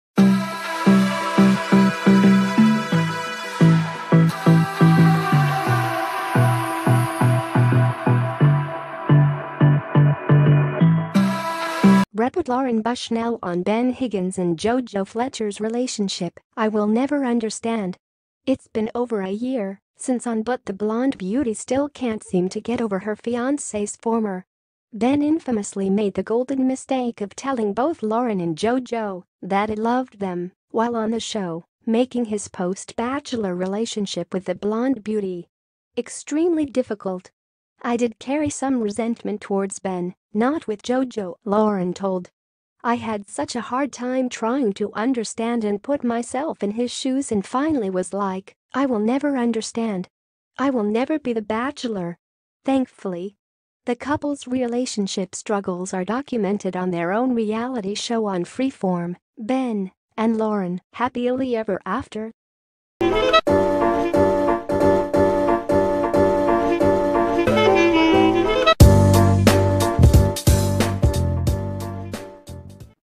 REPORTLauren Bushnell on Ben Higgins and JoJo Fletcher's Relationship: "I Will Never
REPORTLauren Bushnell on Ben Higgins and JoJo Fletcher's Relationship: "I Will Never Understand". It's been over a year since  on , but the blonde beauty still can't seem to get over her fiancé's former .  Ben infamously made the golden mistake of telling both Lauren and JoJo that he loved them while on the show, making his post-Bachelor relationship with the blonde beauty extremely difficult.
  “I did carry some resentment towards [Ben], not with JoJo,” Lauren told . “I had such a hard time trying to understand and put myself in his shoes and finally was like, ‘I will never understand. I will never be The Bachelor. Thankfully.’” The couple's relationship struggles are documented on their own reality show on Freeform, Ben and Lauren: Happily Ever After?.